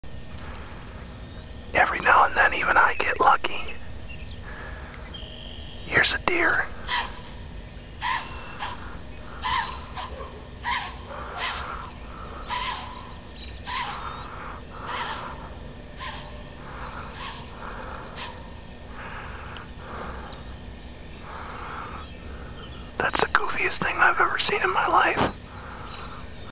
Caught a deer while waiting for M337 6/9/08
This deer was walking up to me as I waited for tonight's M337 to get there. He seemed OK with me, until I started filming him...then he went balistic on me!! I dunno, maybe camera shy??? Can you say boing boing boing~!